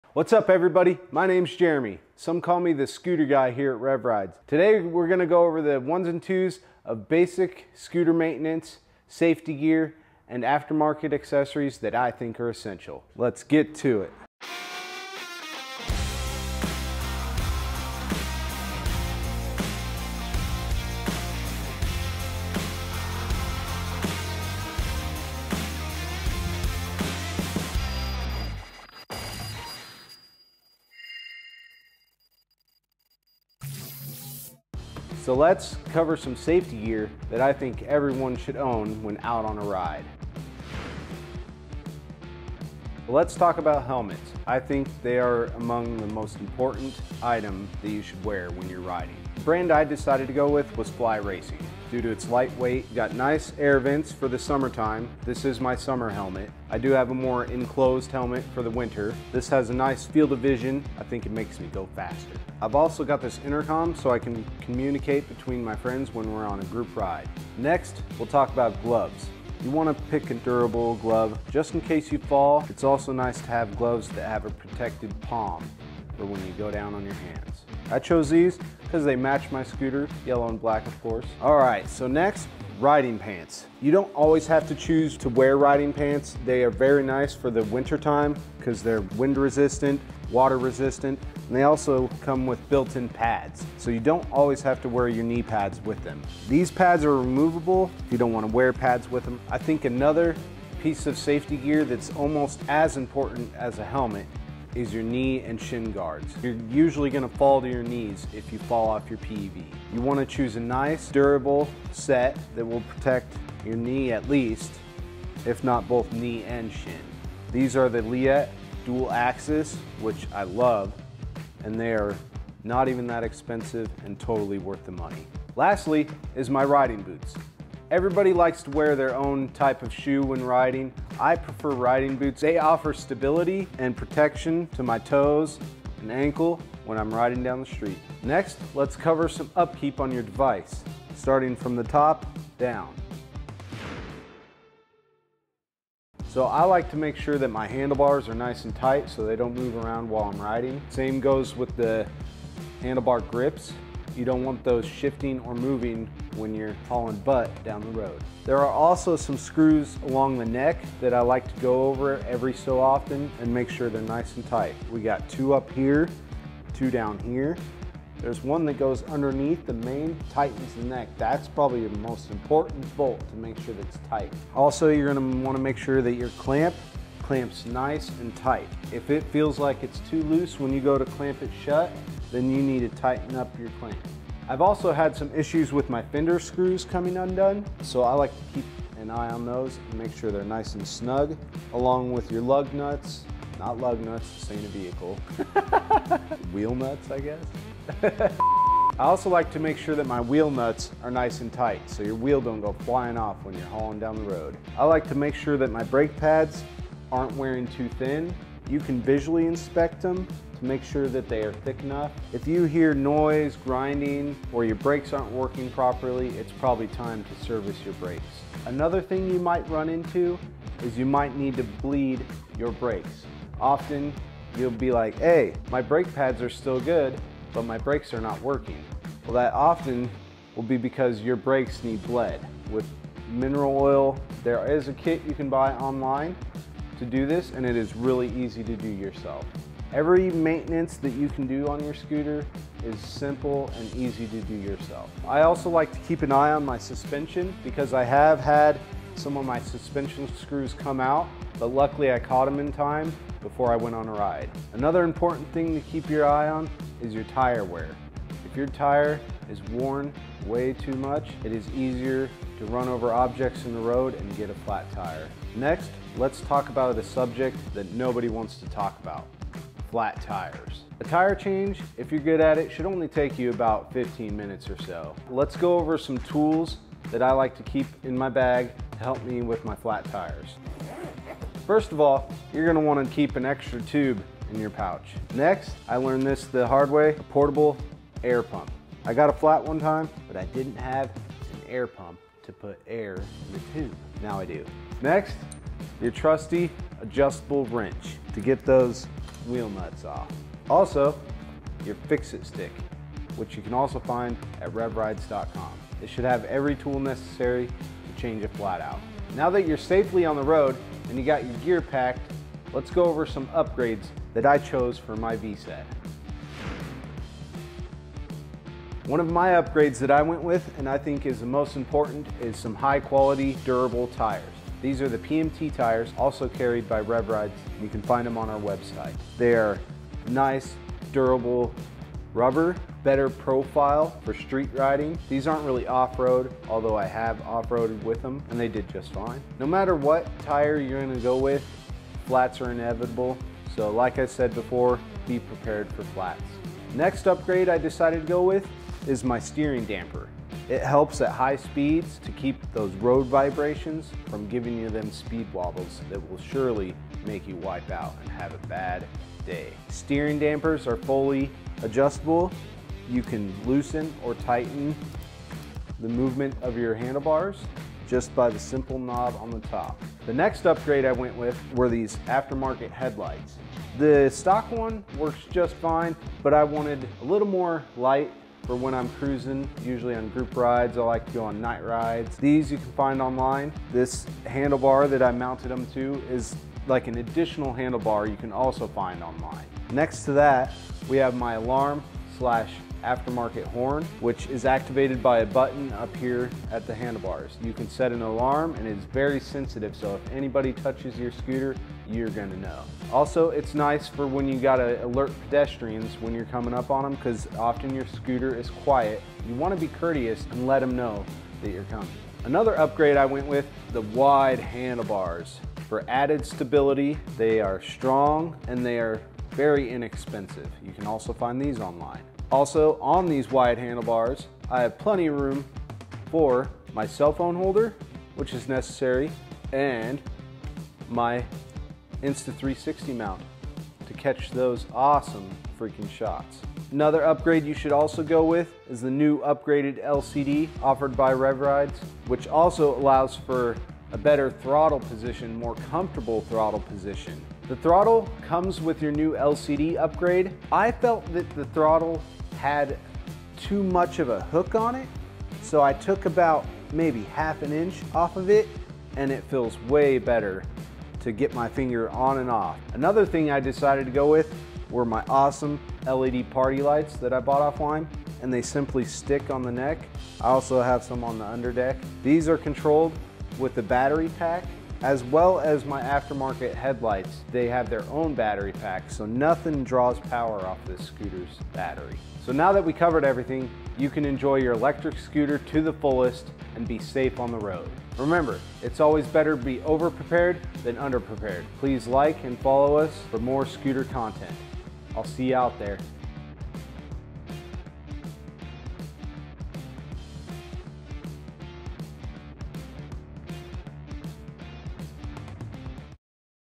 Safety, Maintenance & Accessory Tips for All Electric Scooter Riders
Our scooter guy Jeremy Rogers is here today to give us his insight on how to stay safe, how to maintain your scooter and optimize it's capabilities, and what things can be added on to your scooter to enhance your riding experience!

Fly Racing Full Face Helmet

Wireless Bluetooth Intercom

COFIT Motorcycle Gloves

Waterproof Riding Pants

Leatt Dual Axis Knee/Shin Guards

Riding Boots

Portable Electric Air Pump

Adjustable Wrench

After Market Headlights

Alarm / Aftermarket Horn

Solid Wide Handle Bar